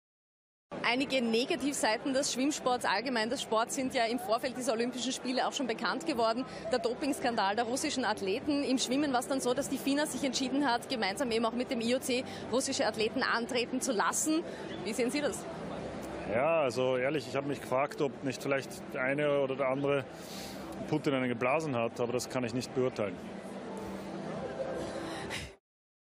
Olympia Rio 2016 - Markus Rogan: Putin Blowjob Sager (Blasen)
Markus Rogan der österreichische Ex-Schwimmer sorgt bei der Olympia Rio 2016 mit seinem provokanten Blowjob Sager Live im ORF1 für Empörung bei den Mitgliedern des internationalem Olympischen-Komitee...